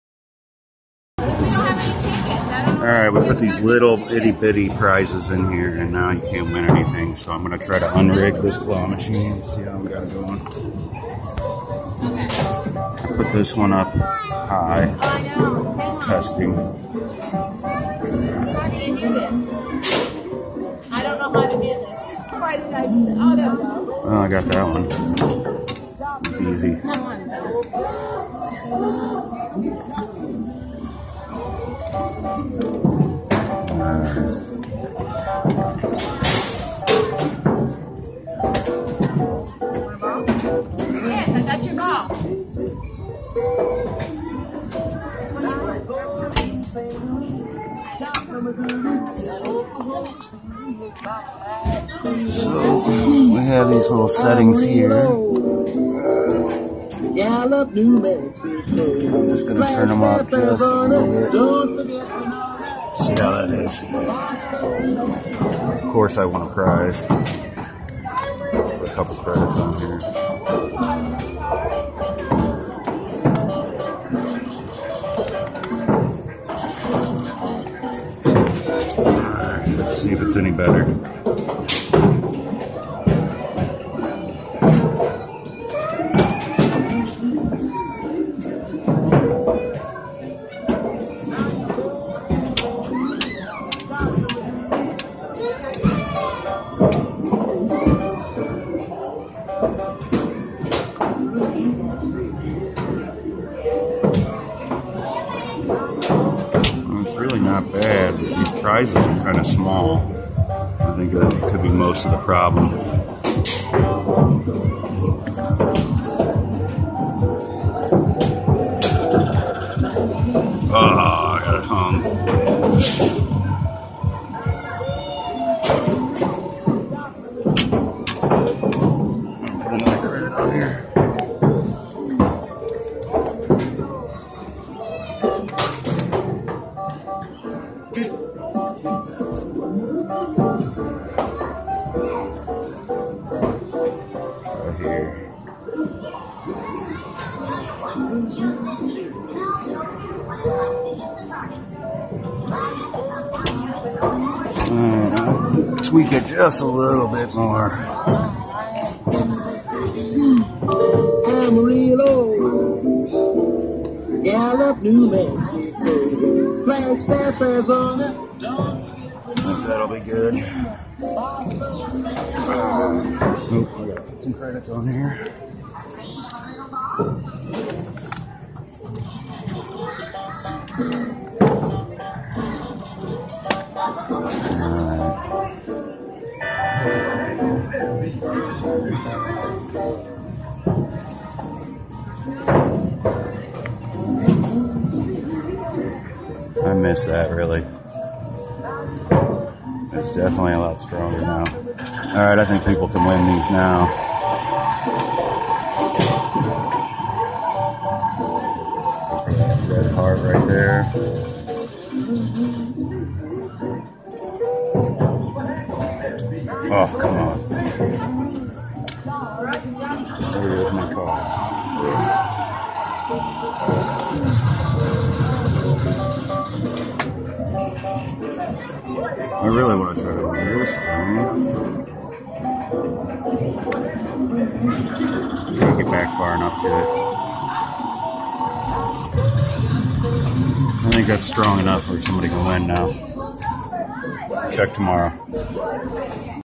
Adjusting Claw Strength on a Claw Machine
The Valentine's Day prizes are much smaller and harder to win, than the previous Christmas prizes. I'm adjusting the strength to make it even possible to win.